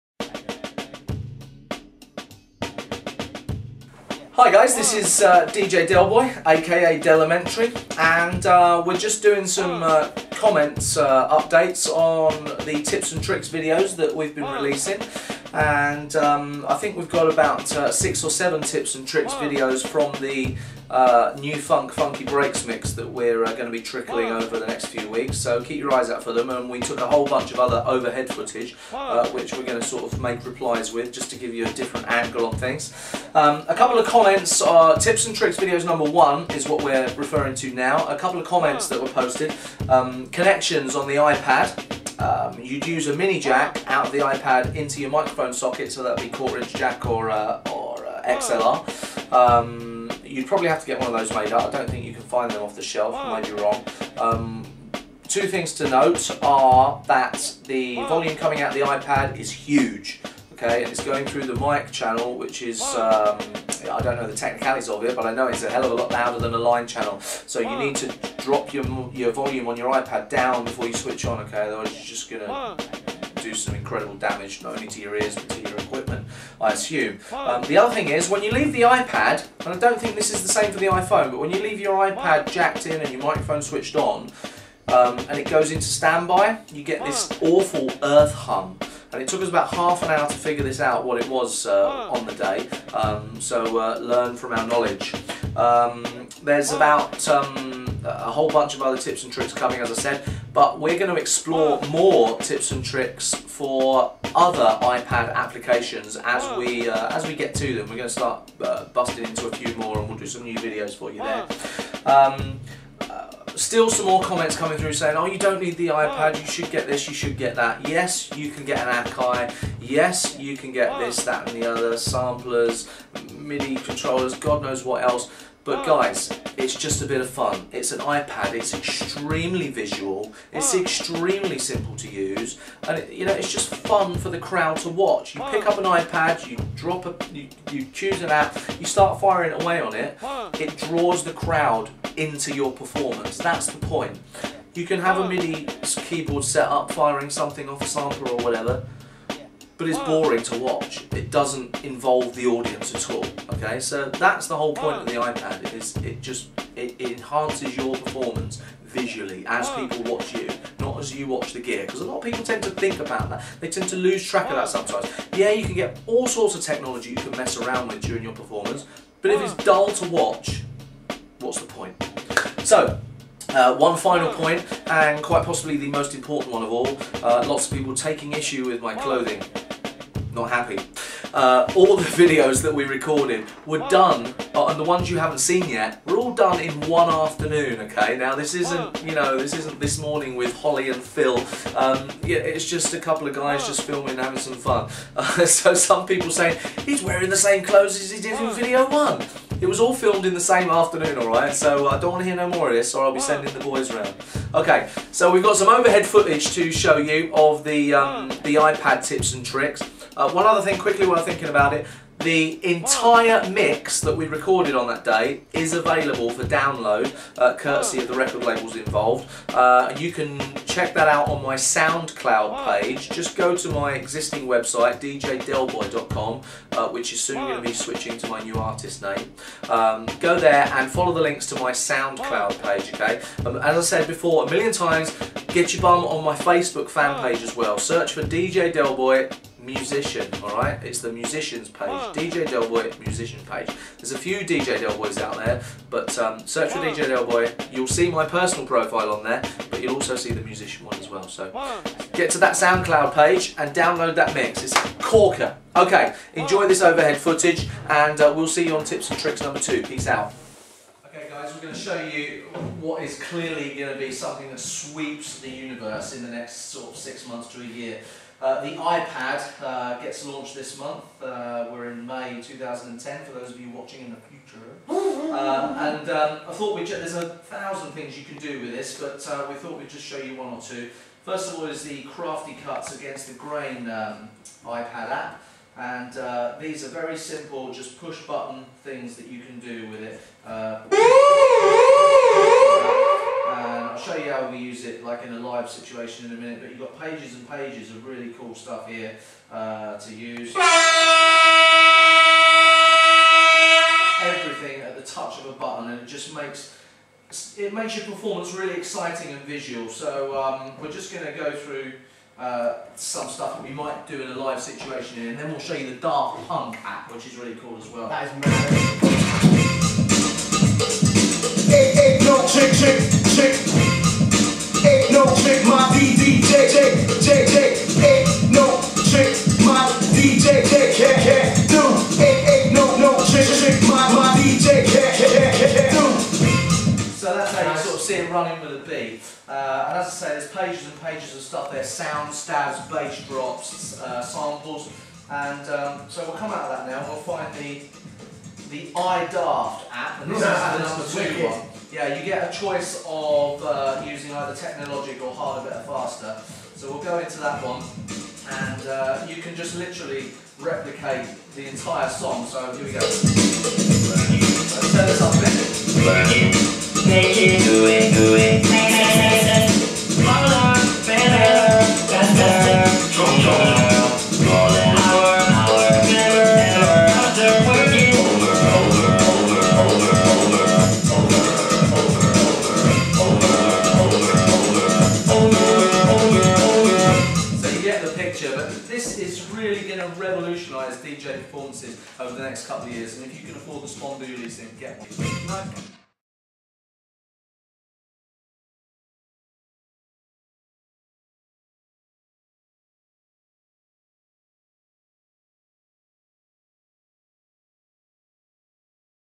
DJ Delboy aka 'Delimentary' NuFunk & Funky Breaks Mix - Ipad/Iphone Tips & Tricks Number 1 Reply
Just wanted to put some thoughts together in response to all the postings that Jonathan aka DJ Tutor / Ellaskins received.
Really appreciate all the props and I hope my videos inspire a lot of you to get creative & explore some funky nu beats. Loads more to come over the next few weeks.